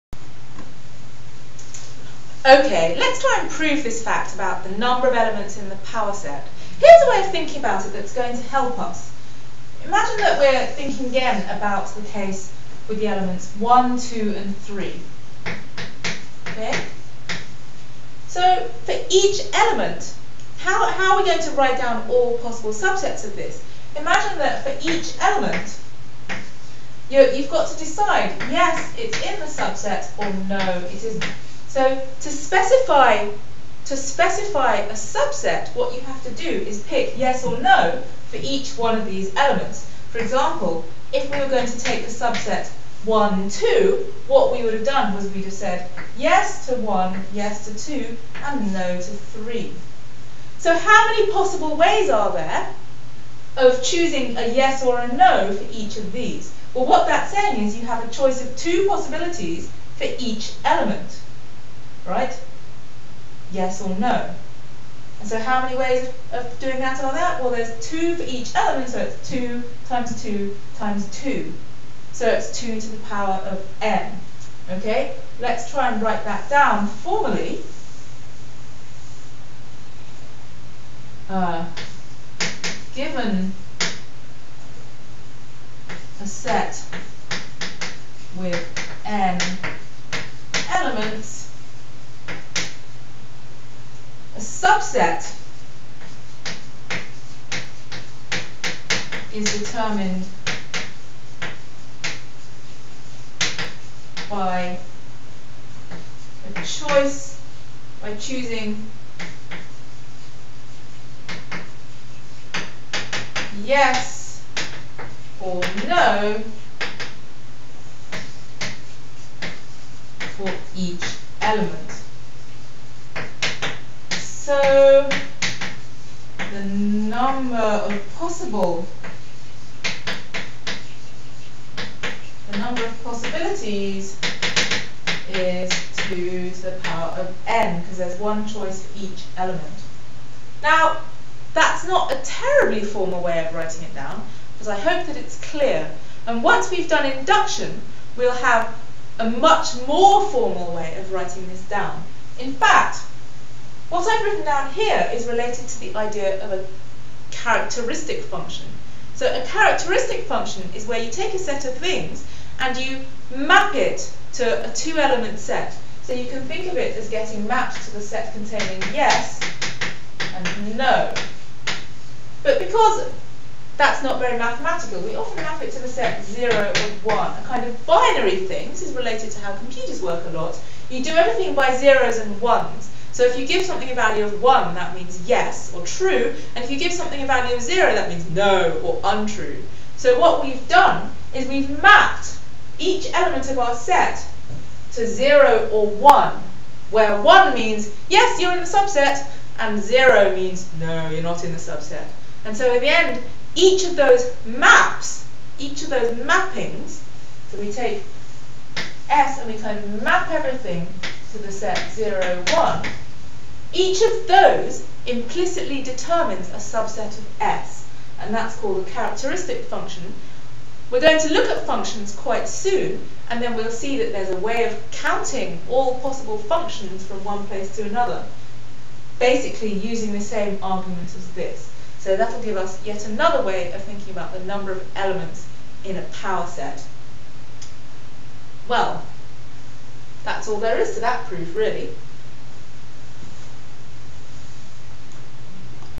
Sets 12
A short explanation of the number of elements in a power set.